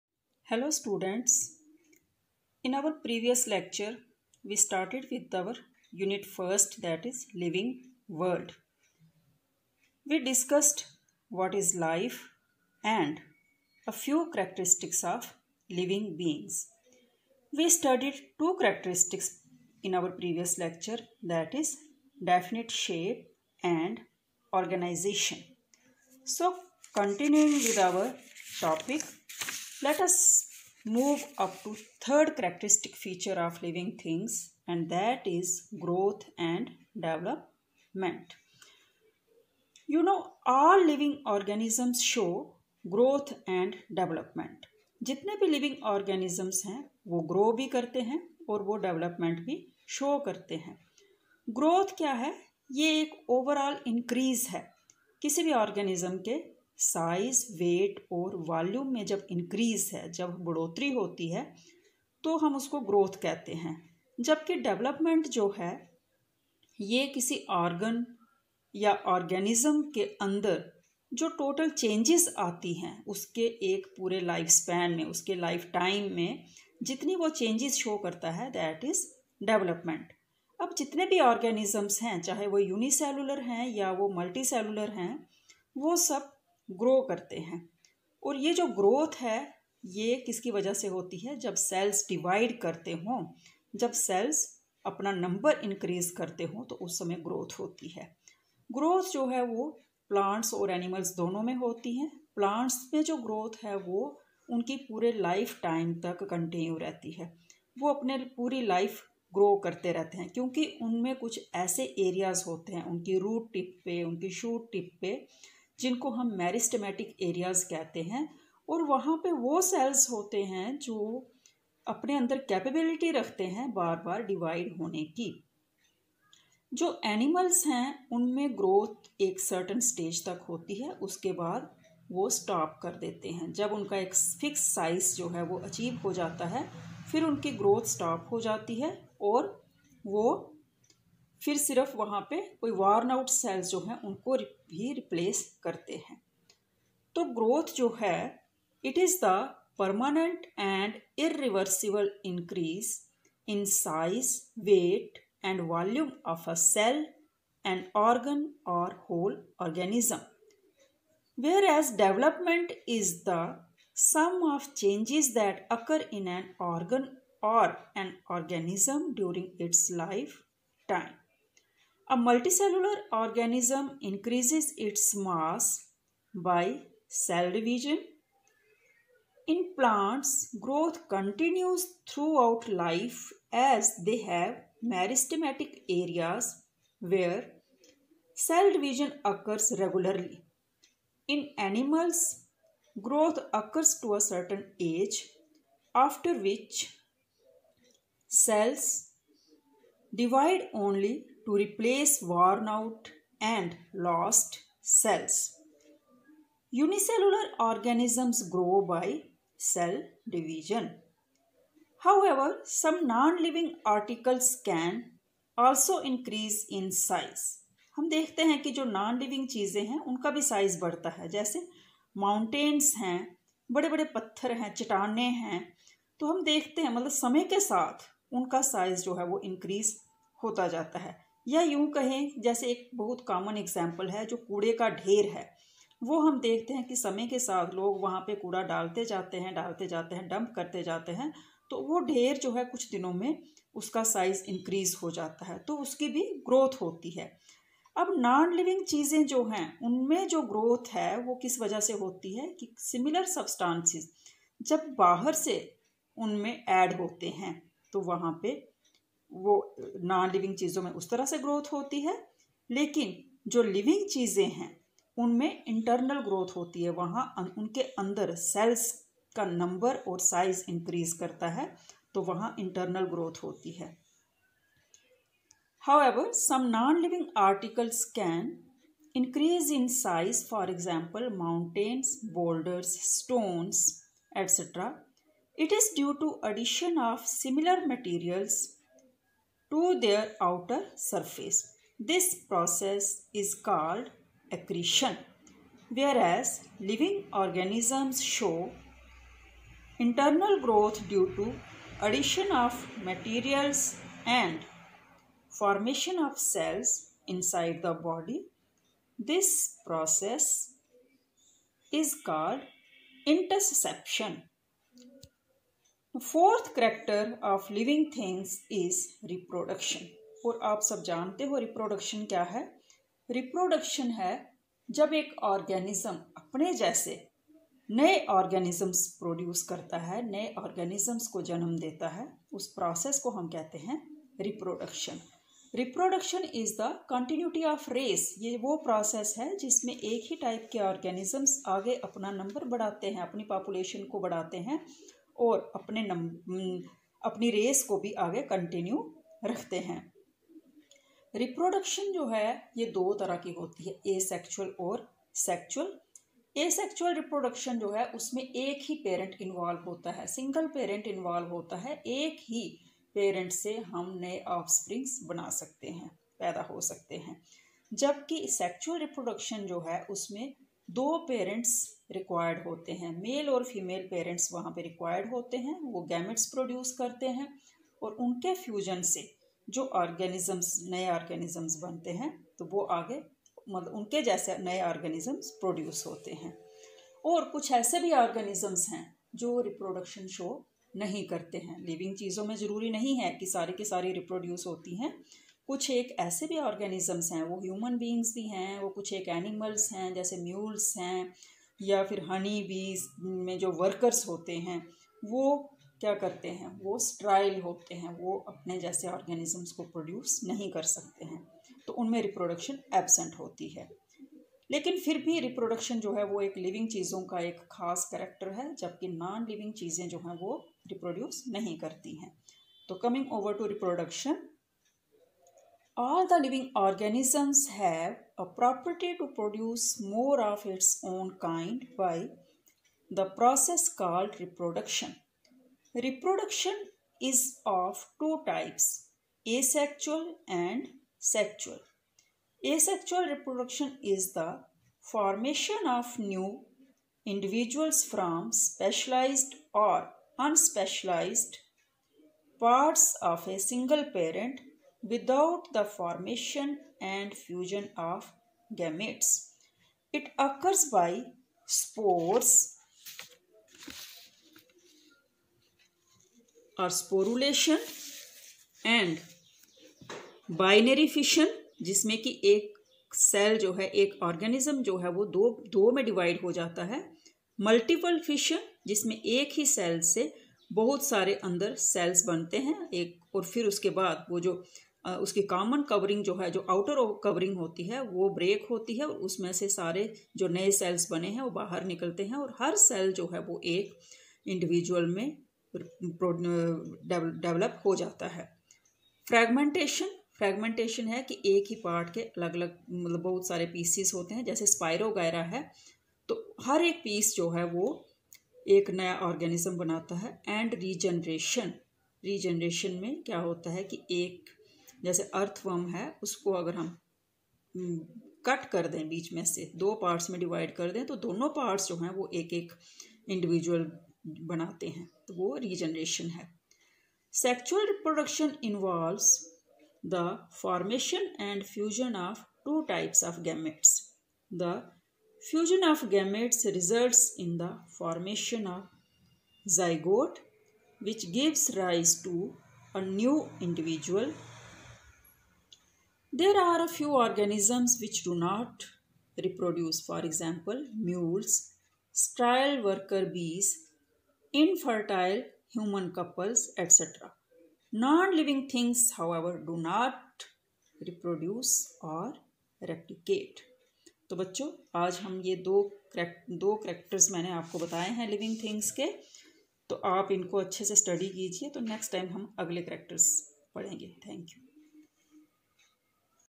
TOPIC: CHARACTERISTICS OF LIVING THINGS, CLASS: XI, BY: SUSHI GUPTA SR. LECTURER, GHSS BOYS UDHAMPUR
TOPIC: CHARACTERISTICS OF LIVING THINGS, GROWTH & DEVELOPMENT, REPRODUCTION
CLASS: XI,
BY: SUSHI GUPTA SR. LECTURER, GHSS BOYS UDHAMPUR